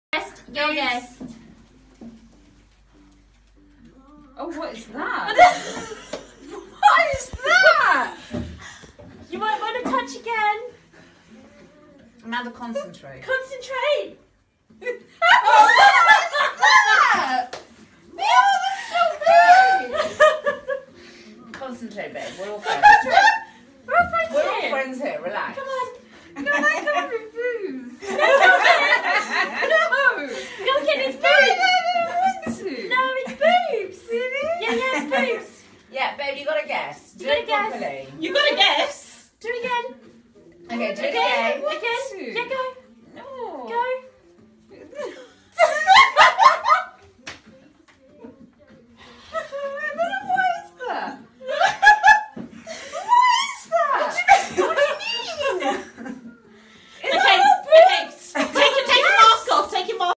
Breast who? Hen party games
The best way to introduce a Butler in the Buff to a party!
Butlers are  great party accessory